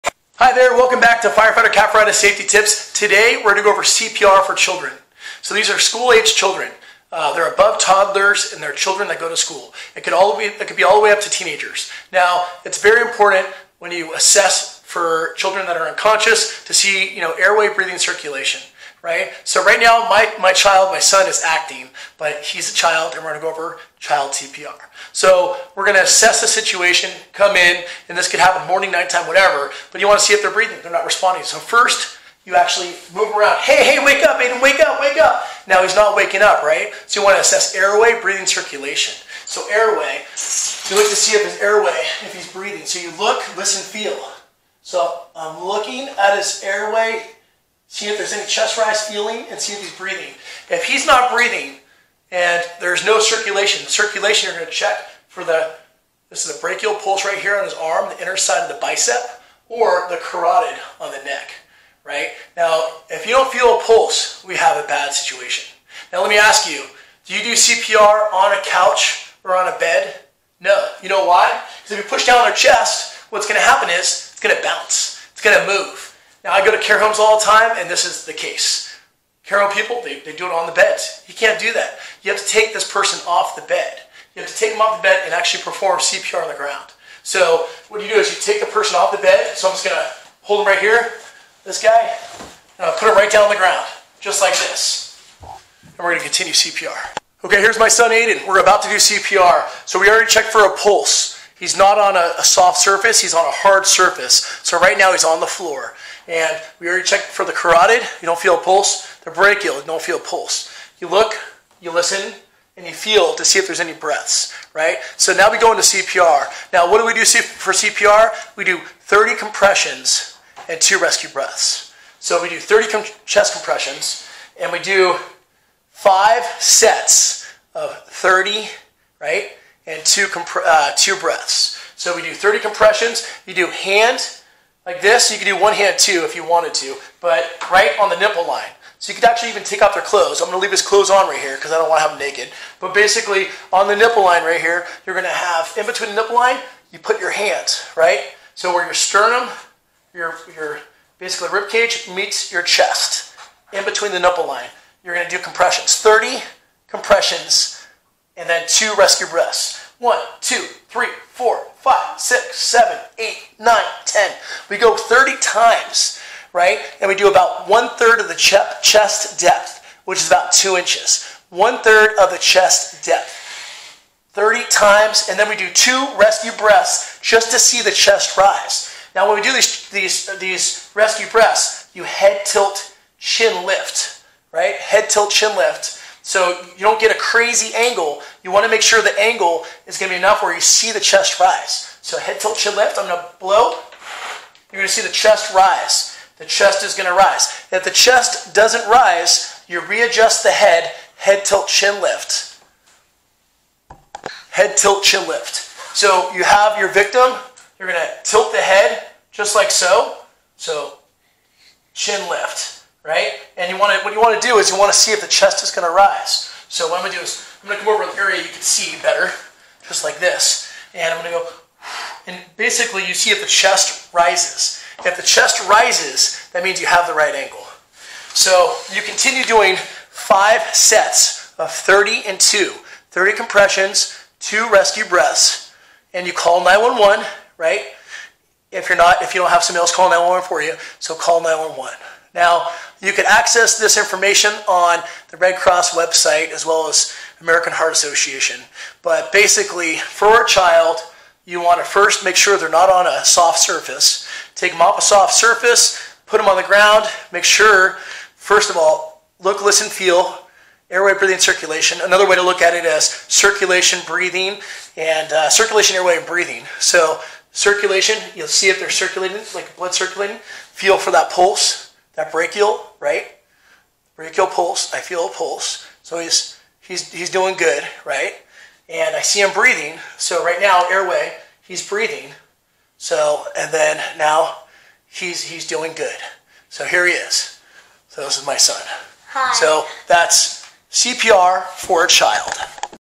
Know - CPR for children!
Instructions on how to perform CPR on a child. Remember please don't call me in an emergency situation but call emergency service (911). My video is for reference and for further information and resources look on the American Red Cross website and American Heart Association website.